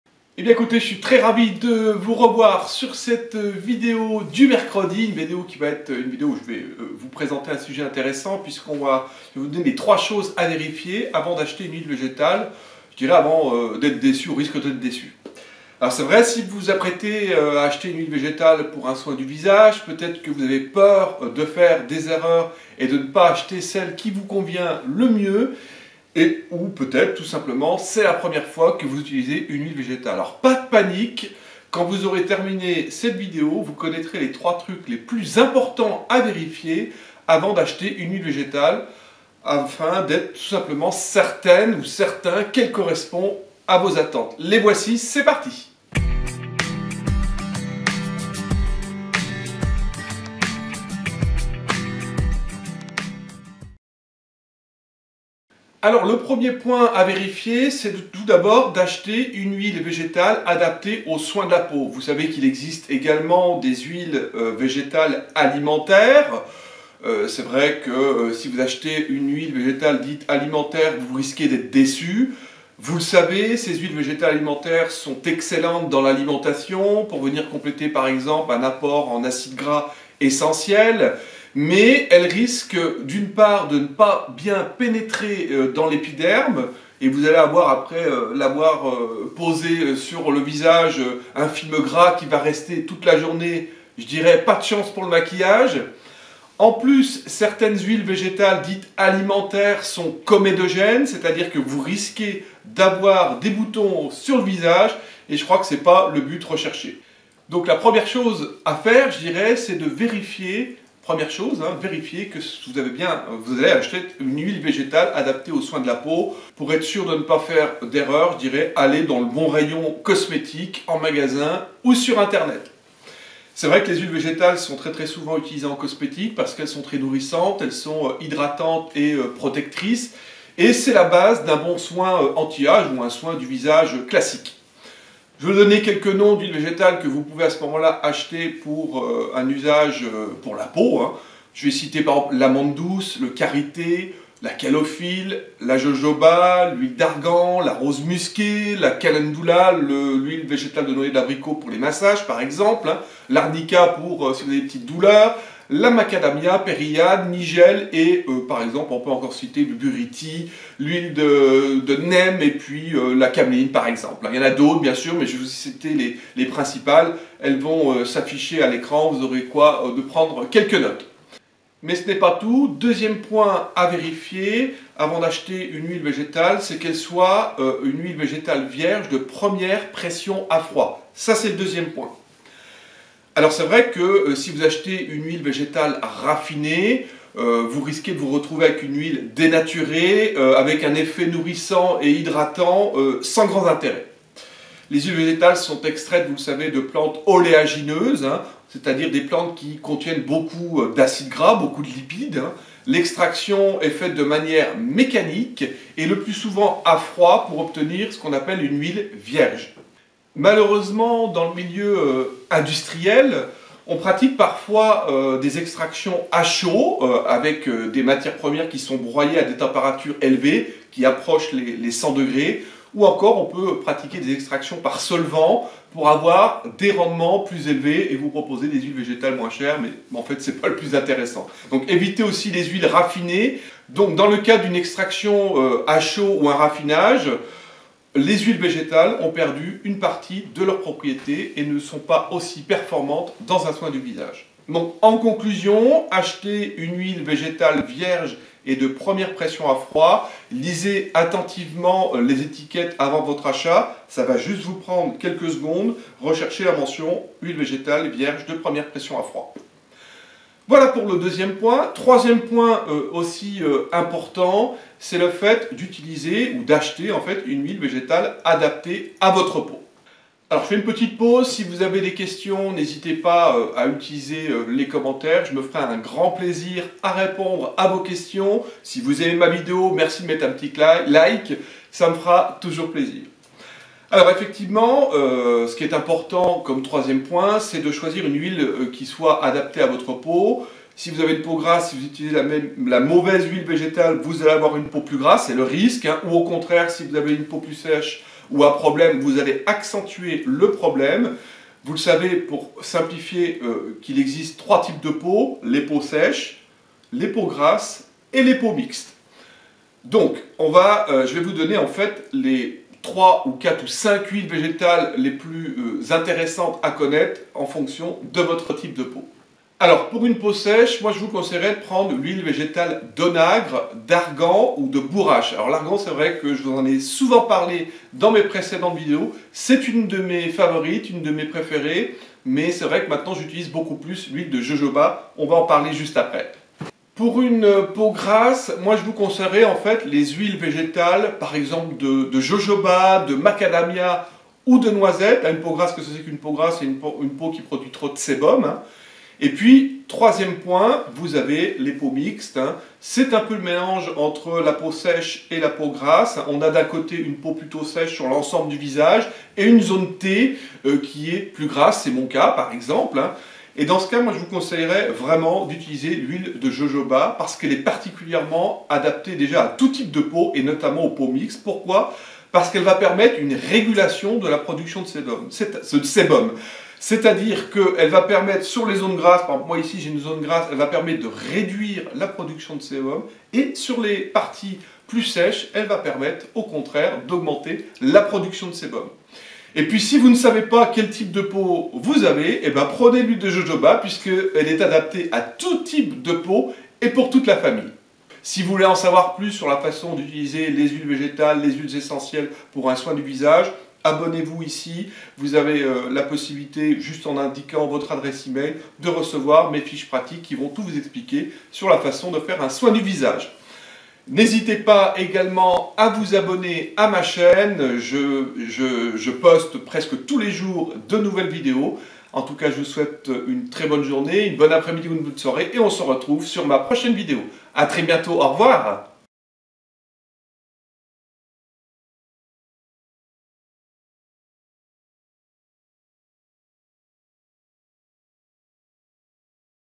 3 choses à vérifier avant d'acheter une huile végétale (au risque d'être déçue).
Vous voulez acheter une huile végétale ? Vous voulez être certain qu'elle corresponde à vos attentes ? Vous ne voulez pas faire d'erreur ?

Dans cette vidéo, je vous expique les 3 choses importantes à vérifier avant d'acheter une huile végétale.